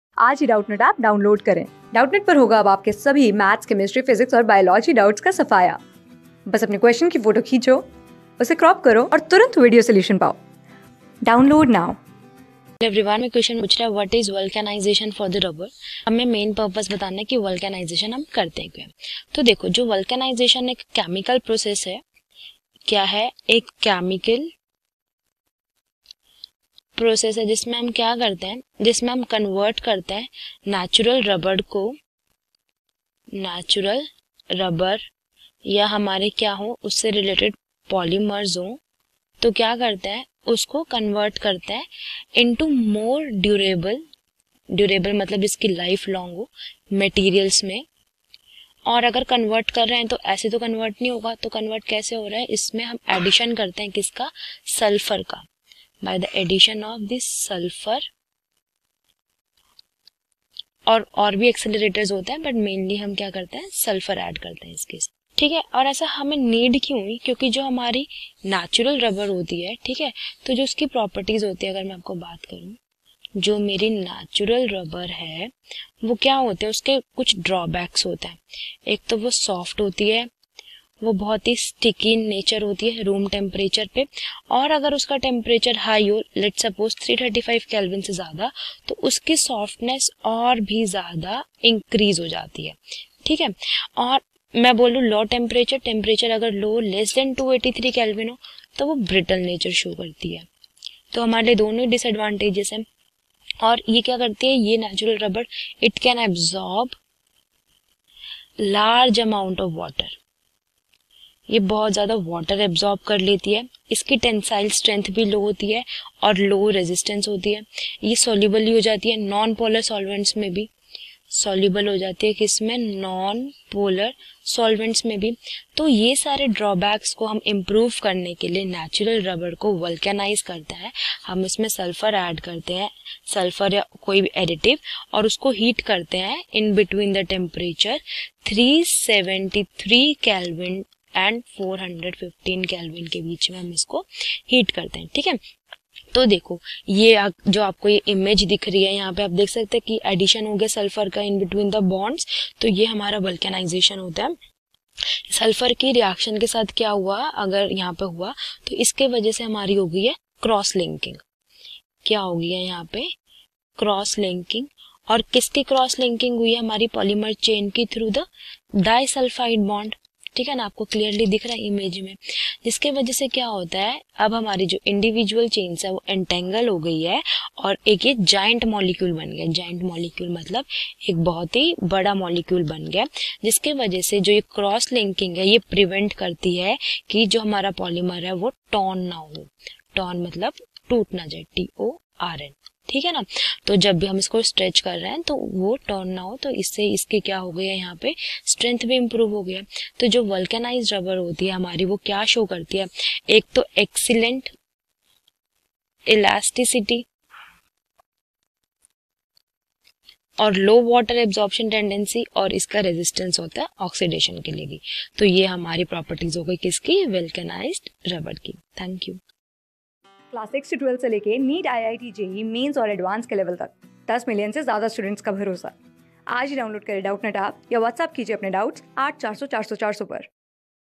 What is vulcanizationfor rubber ? Discusses the main purposeof vulcanizationof rubber | CLASS 12...
What is vulcanizationfor rubber ? Discusses the main purposeof vulcanizationof rubber
Class: 12
Subject: CHEMISTRY
Chapter: CHEMISTRY IN EVERYDAY LIFE
Board:CBSE

👉 Have Any Query? Ask Us.